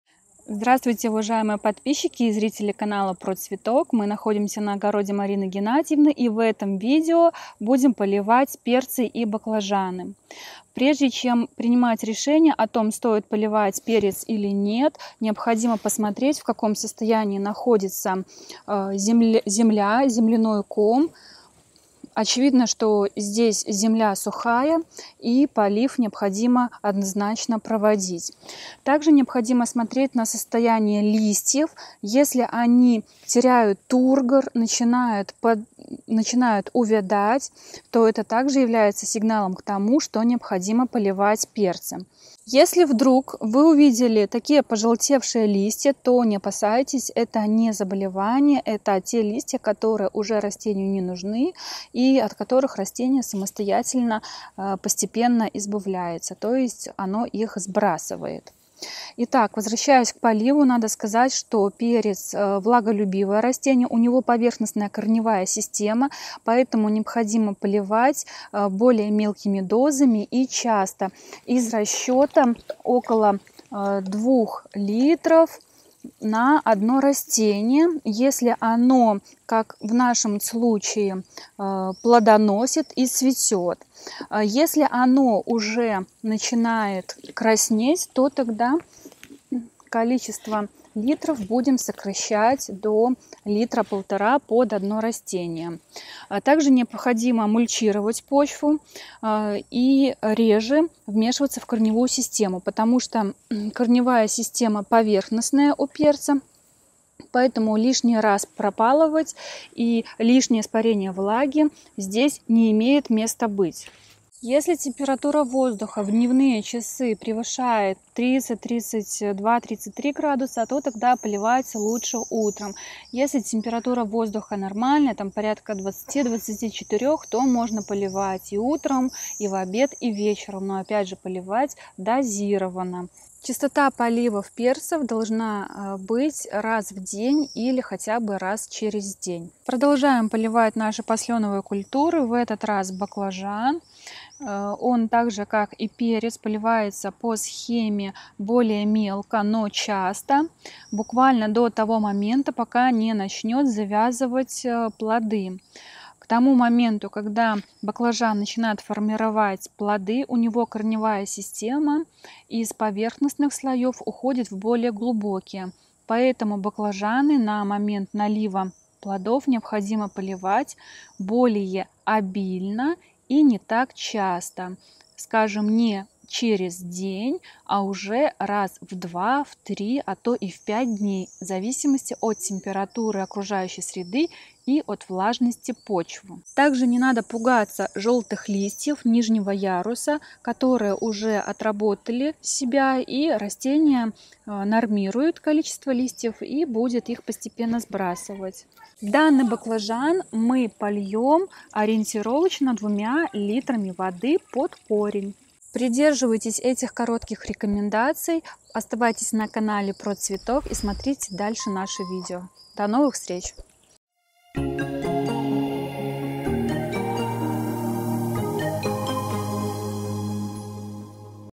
ПОЛИВАЙТЕ ПЕРЦЫ И БАКЛАЖАНЫ ТОЛЬКО ТАК! Актуальные рекомендации для большого урожая!
Как правильно поливать перцы и баклажаны в середине лета, когда растения вовсю цветут и завязывают плоды? Почему не стоит переживать из-за пожелтения нижних листьев? Как меняется потребность в воде у растений в зависимости от фазы развития? Обо всем этом коротко, экономя ваше время в горячую пору, рассказывает Анна Пашкевич, научный сотрудник РУП «Институт овощеводства». С советами автора вашим растениям будет всегда достаточно влаги, и они порадуют вас отменным урожаем.

00:00 Вступление
00:10 Как понять, что перцам нужен полив – по состоянию земли и листьев
00:45 Почему пожелтение и отмирание нижних листьев у перцев – не проблема
01:00 Перец – как правильно поливать
01:40 Перец не любит беспокойства корневой системы
01:55 Как поливать перцы в жаркую погоду
02:13 Как часто надо поливать перцы
02:20 Как правильно поливать баклажаны во время цветения и плодоношения
03:06 Почему баклажан сбрасывает нижние листья
03:18 Объем воды для полива баклажана
03:25 Заключение